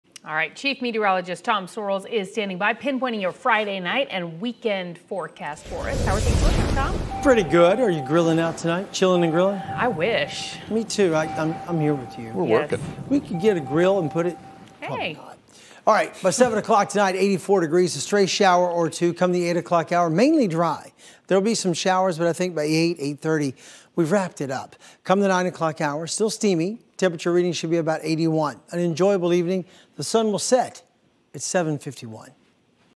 Grill & Chill forecast -- 8/28/20
It'll be mainly dry tonight in Central Florida.